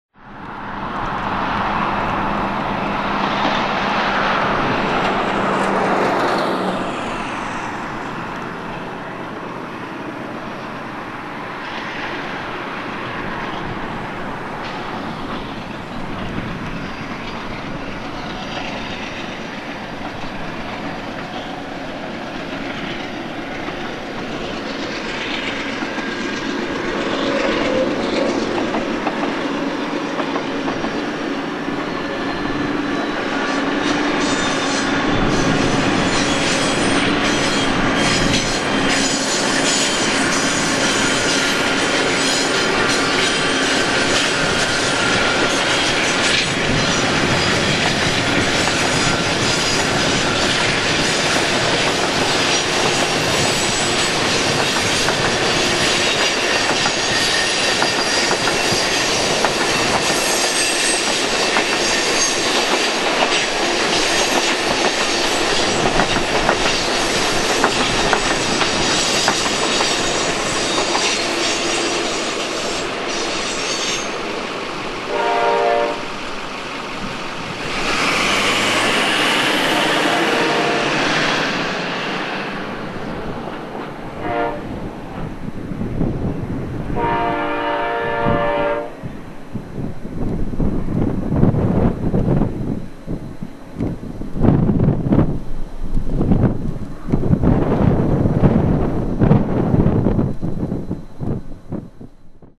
Detoured Amtrak Coast Starlight #11 Through Altamont Pass
The Starlight was detoured again and headed through Altamont Pass area with #129 leading and with a F59PHI Cascades engine second and a parlour car that was on the rear.  Enjoy!!  Shot 9/14/11